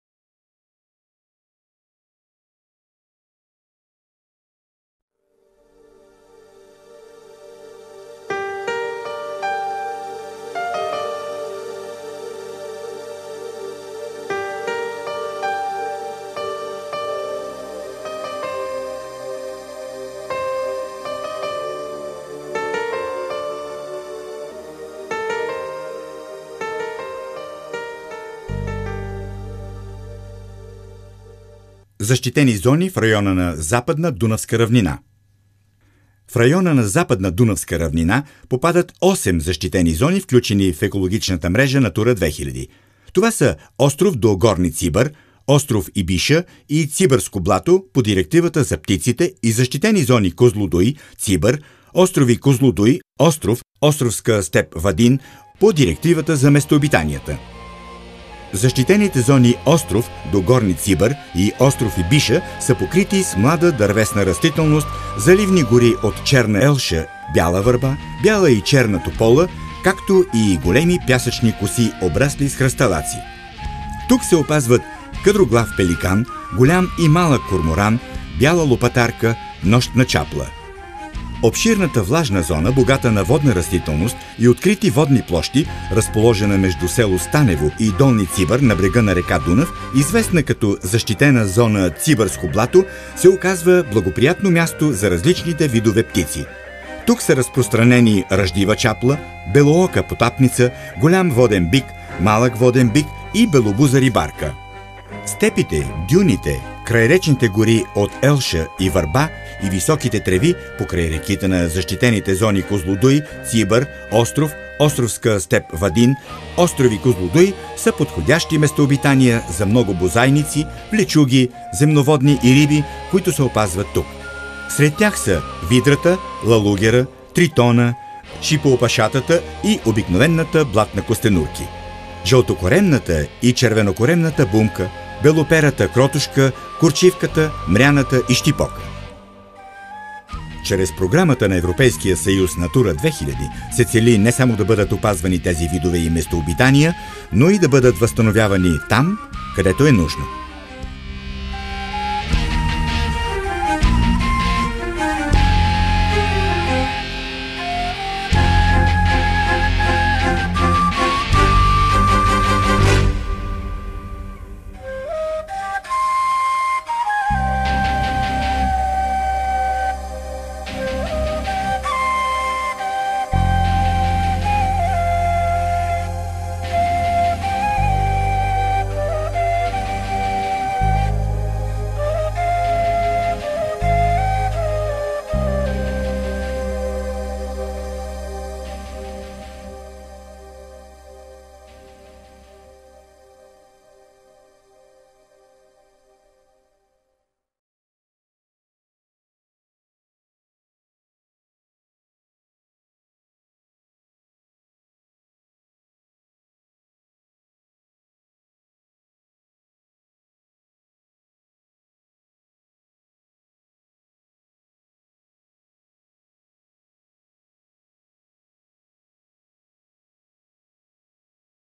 Западна Дунавска равнина: Натура 2000
Остров на р.Дунав, покрит с млада дървесна растителност,  големи пясъчни коси с върбови храсталаци